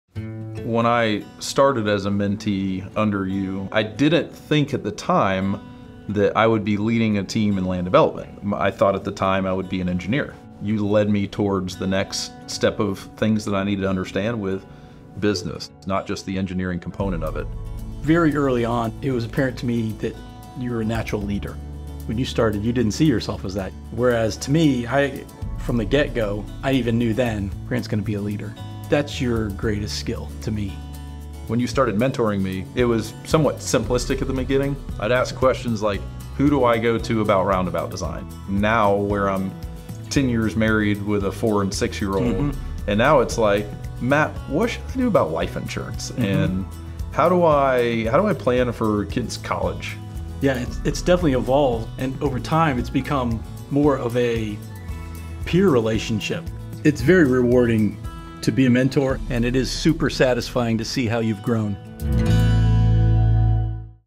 Mentorship at ETM
With an average tenure of seven years and climbing, our employees choose to build a career with ETM for many reasons, but one of the most impactful is our commitment to career development through mentorship. Grant Bledsoe, P.E., and Matthew Maggiore offer some insight into their mentor/mentee relationship and how they have supported each other both professionally and personally.